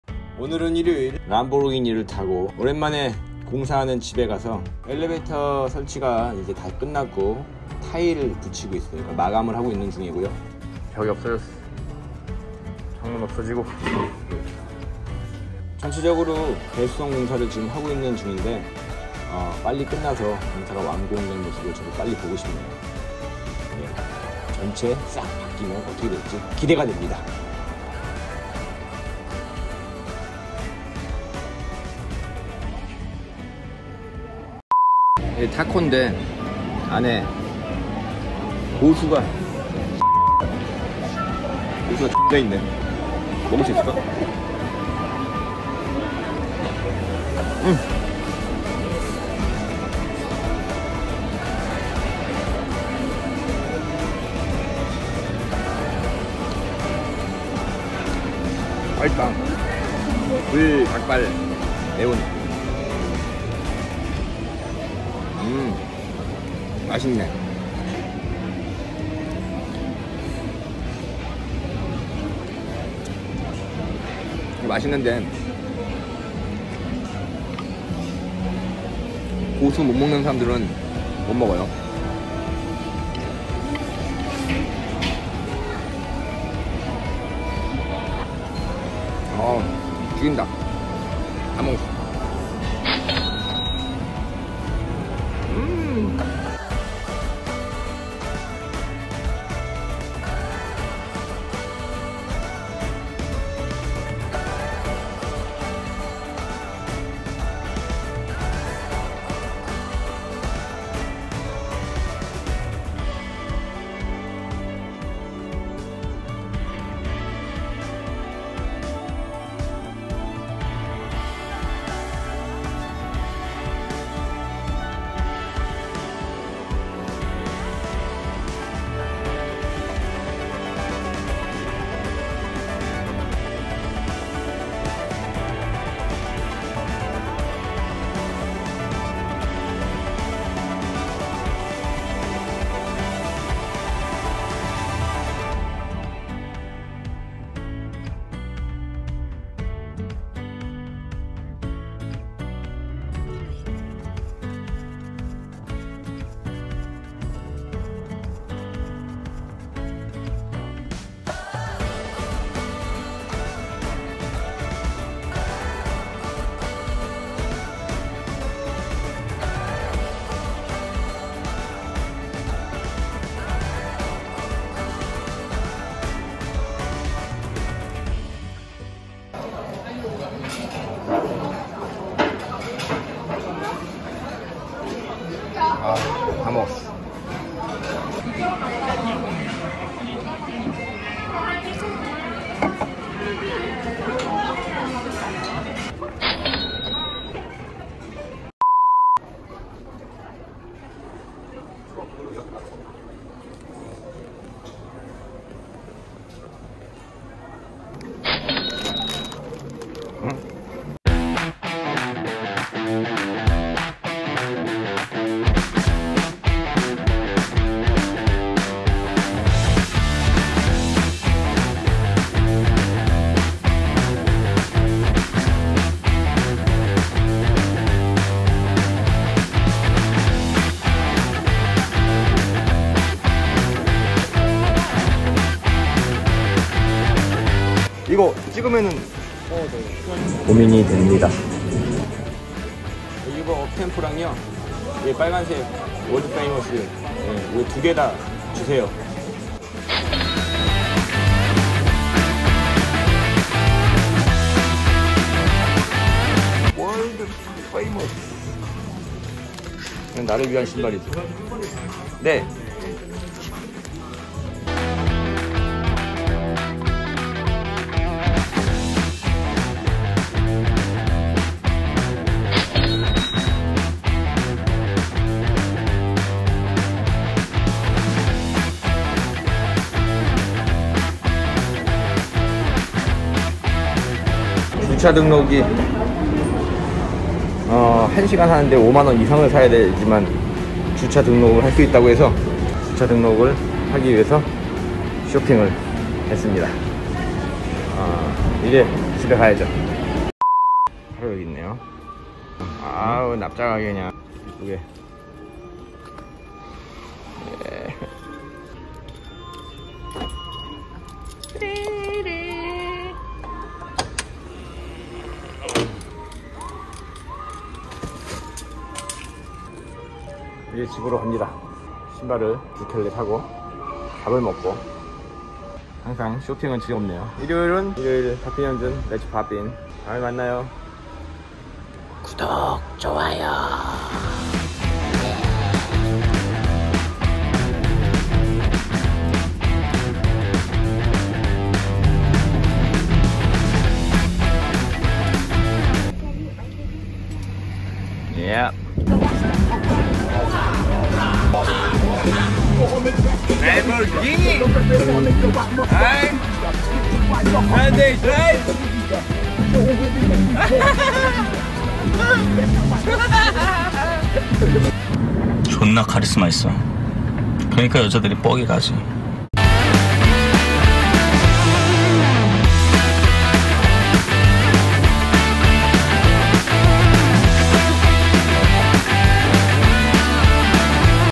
일요일엔 레츠팝핀 / 더현대 주차비를 아끼는 방법 ~ 지하 음식점중 맛집을 소개합니다 한정판 나이키신발들과 꿀정보 가득 팝핀현준의 레츠 팝핀!!
더현대 에 다녀왔습니다